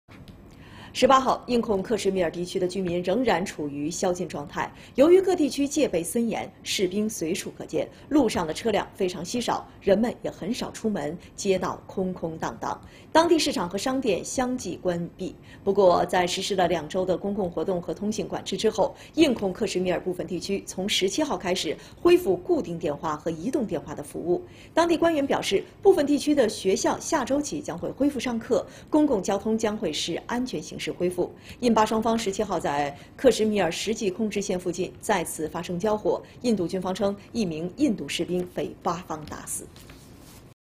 [中国新闻] 印控克什米尔放松部分管制 | CCTV中文国际
印控克什米尔放松部分管制。